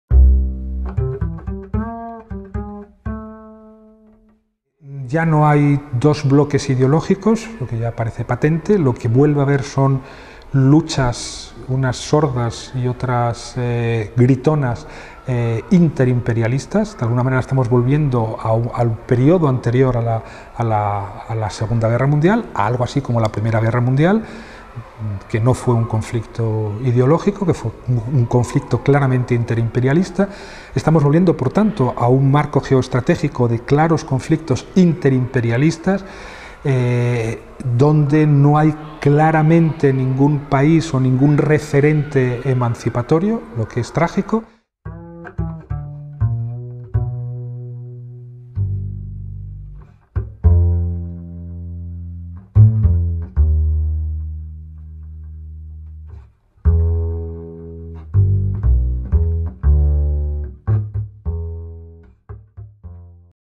Estamos volvendo a un marco xeoestratéxico de loitas interimperialistas
Durante as xornadas “Medios, comunicación e poder”, Galiza Ano Cero tivo oportunidade de conversar co filósofo e ensaísta Santiago Alba Rico (Madrid, 1960).

Durante o primeiro bloque da conversa, en base ás experiencias recollidas na súa residencia en Túnez, expón as súas impresións sobre os particulares 'días que estremeceron o mundo' desde o Magreb durante os anos 10 do presente século.

Na segunda, a través da súa experiencia como analista en 'Rebelión', analiza os conceitos cidadanía, democracia e a criminalización dos movimentos sociais.

galizaanocero.tv